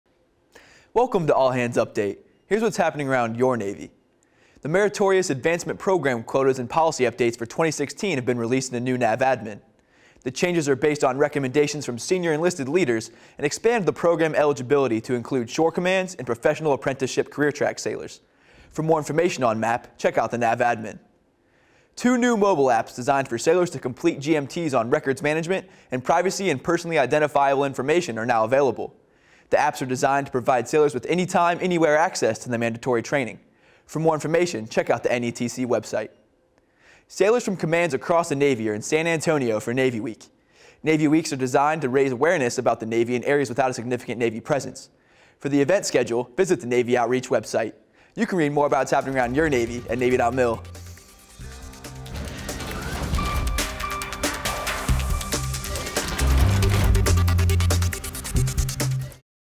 Headlines for Wednesday, April 20, 2016 (HL-20)
Updates for meritorious advancement program. New GMT Mobile Apps. Navy week San Antonio.